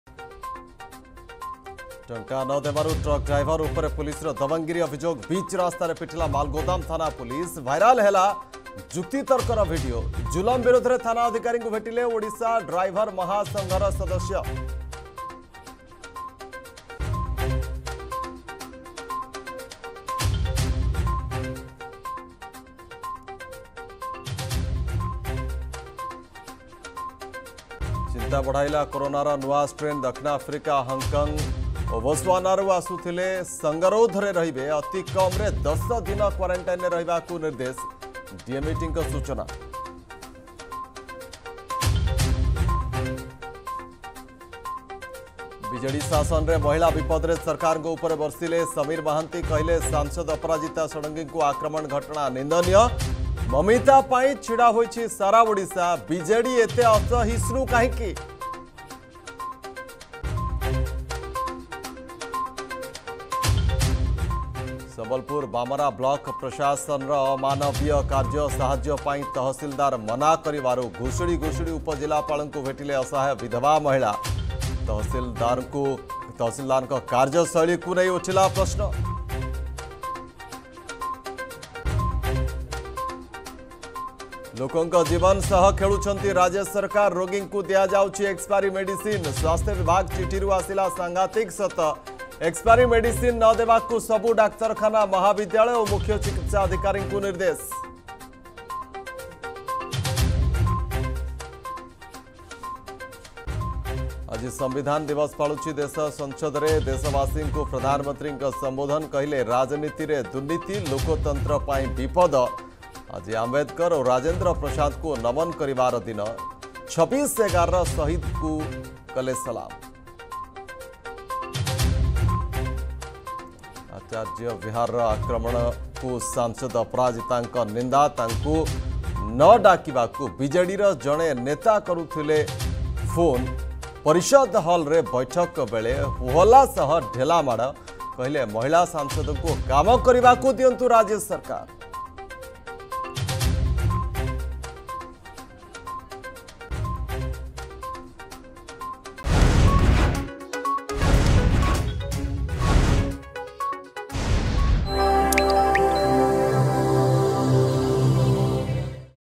Headlines 2pm | 26 Nov 2021 | Argus News
+ଟଙ୍କା ନ ଦେବାରୁ ଟ୍ରକ ଡ୍ରାଇଭର ଉପରେ ପୋଲିସର ଦବଙ୍ଗଗିରି ଅଭିଯୋଗ
+ଚିନ୍ତା ବଢ଼ାଇଲା କରୋନାର ନୂଆ ଷ୍ଟ୍ରେନ୍
+ବିଜେଡି ଶାସନରେ ମହିଳା ବିପଦରେ
+ସମ୍ବଲପୁର ବାମରା ବ୍ଲକ ପ୍ରଶାସନର ଅମାନବୀୟ କାର୍ଯ୍ୟ
+ଲୋକଙ୍କ ଜୀବନ ସହ ଖେଳ ଖେଳୁଛନ୍ତି ରାଜ୍ୟ ସରକାର
+ସଂସଦରେ ଦେଶବାସୀଙ୍କୁ ପ୍ରଧାନମନ୍ତ୍ରୀଙ୍କ ସମ୍ବୋଧନ
+ଆଚାର୍ଯ୍ୟ ବିହାରରେ ଆକ୍ରମଣକୁ ସାଂସଦ ଅପରାଜିତାଙ୍କ ନିନ୍ଦା